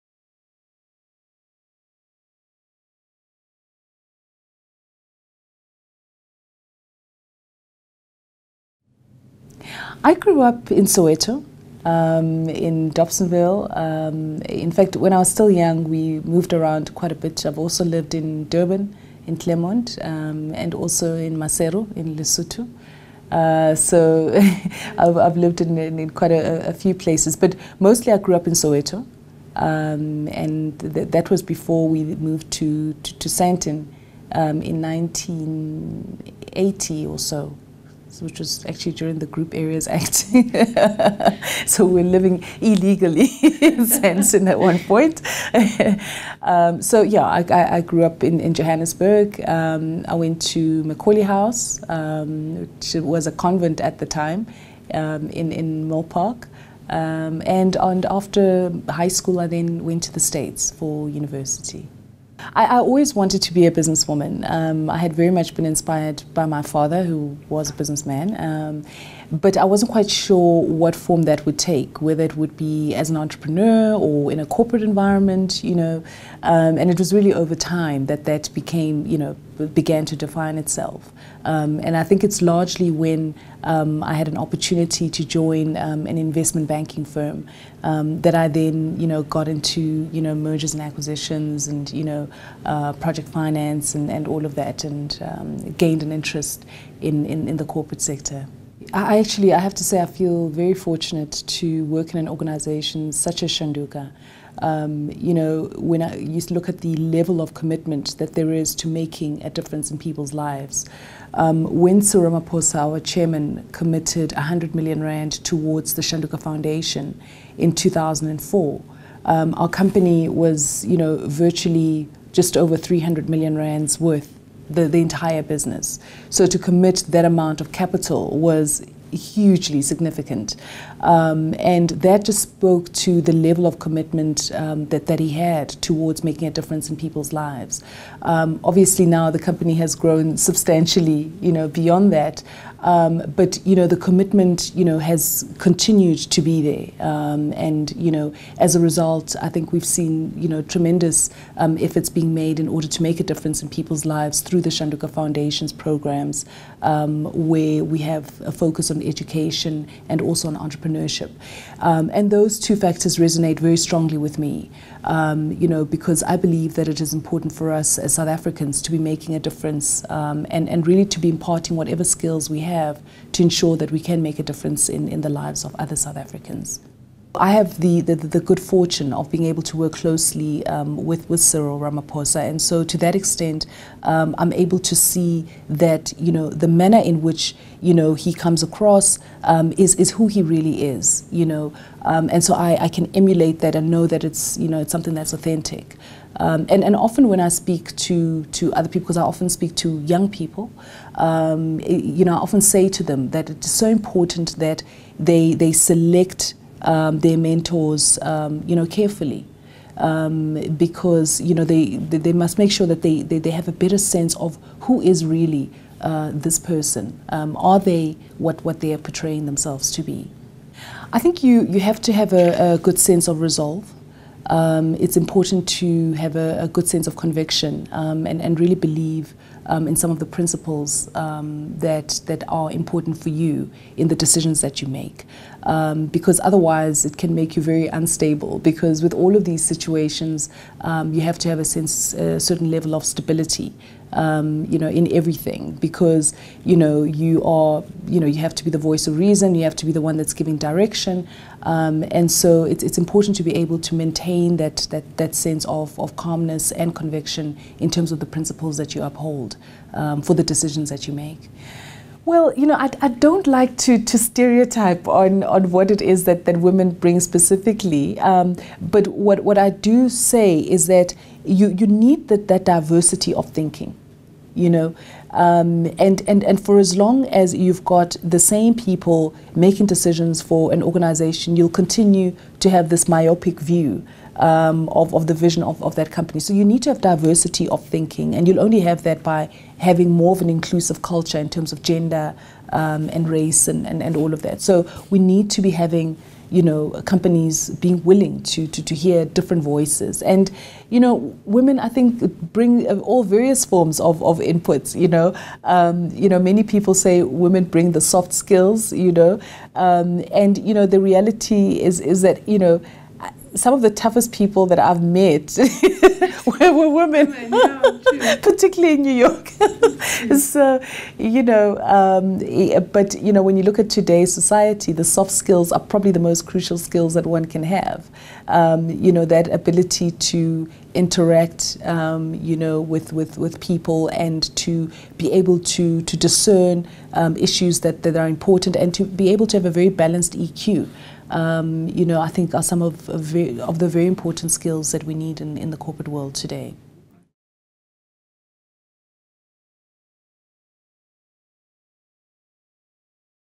Phuti Mahanyele, Shanduka Group CEO , talks to Cape Media and BN Radio
Phuti Mahanyele, Shanduka Group , talks to Cape Media and BN Radio. Awarded as the most influential woman in government and business by financial services in 2009, and listed as one of the top 50 women in the world to watch in 2008 by the Wall Street Journal, Chief Executive Officer at Shanduka Group, Phuti Mahanyele, is one of South Africa's most respected persona in the corporate world.